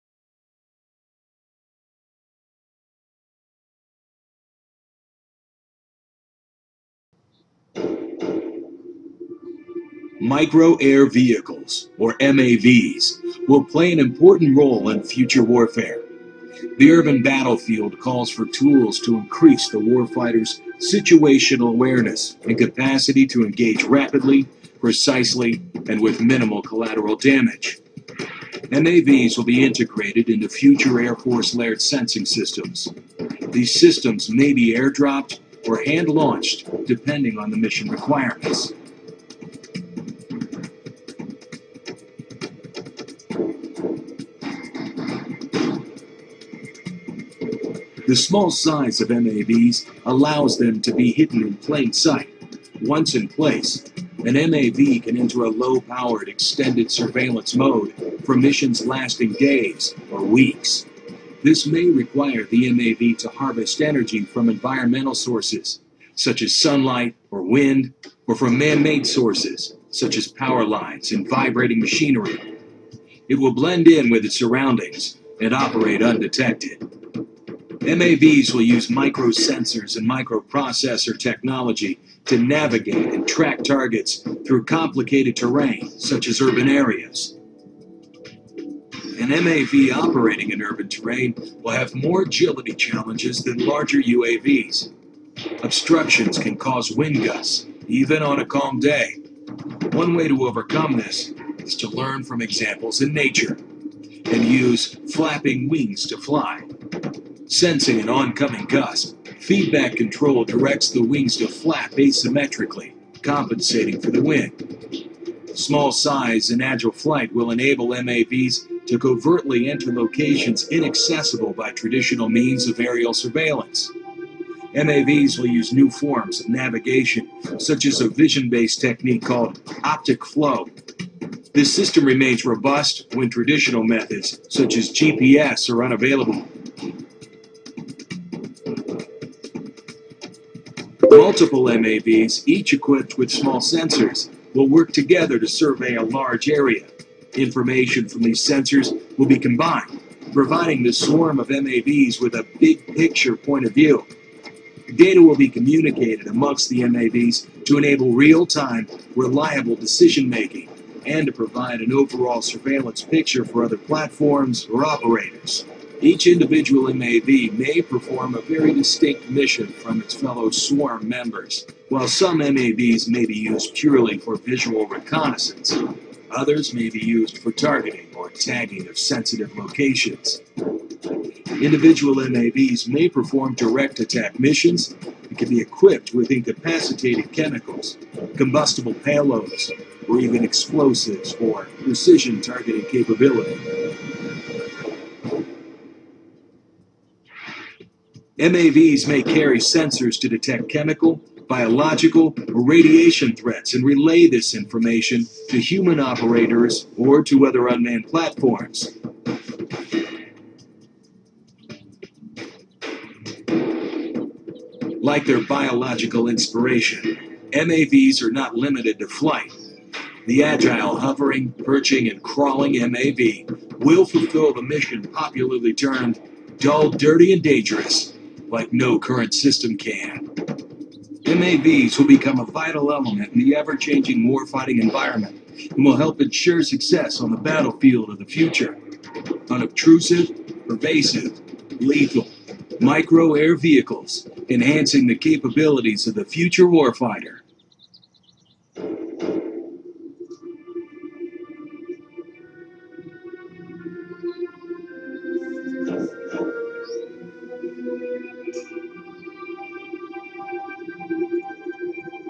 USA Air Force - M.A.V. Micro Air Vehicles
Death from a swarm of tiny drones: U.S. Air Force releases terrifying video of tiny flybots that can can hover, stalk and even kill targets
Air Vehicles Directorate at Wright-Patterson Air Force Base in Dayton, Ohio, is already developing prototypes of tiny drones that can hover
The Micro Air Vehicles will work in swarms to provide complex surveillance of a battlefield
They can also be armed with incapacitating chemicals, combustible payloads or even explosives 'for precision targeting capability'